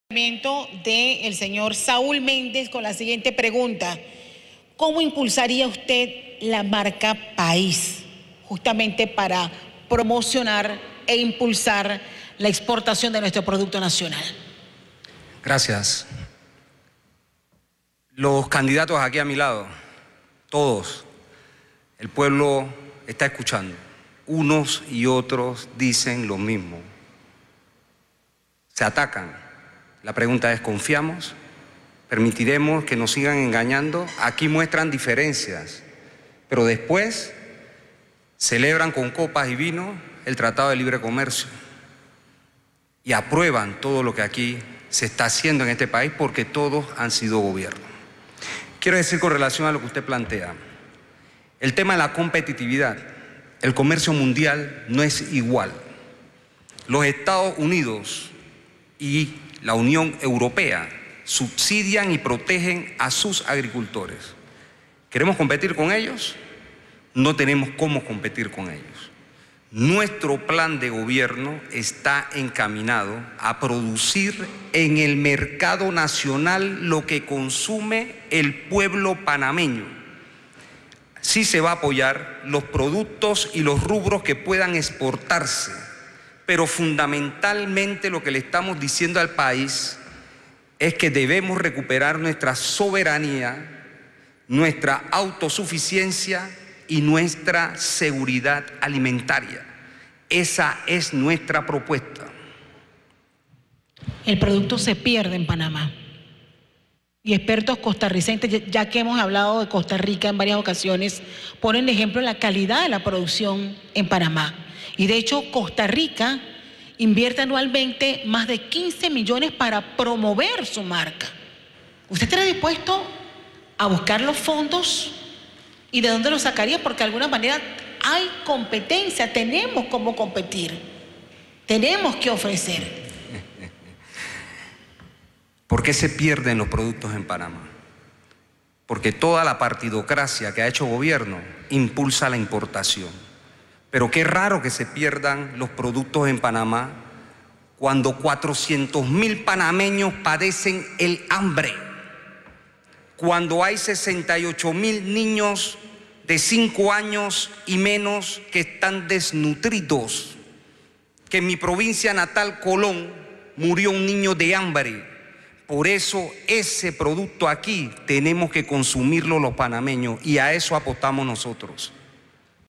3 Saúl Méndez R Primer debate presidencial del sector Agropecuario
Saúl Méndez R. Primer debate presidencial del sector Agropecuario, Tierras Altas Chiriquí
Candidato del pueblo y de Manos Limpias
POR UN PANAMÁ PARA TODOS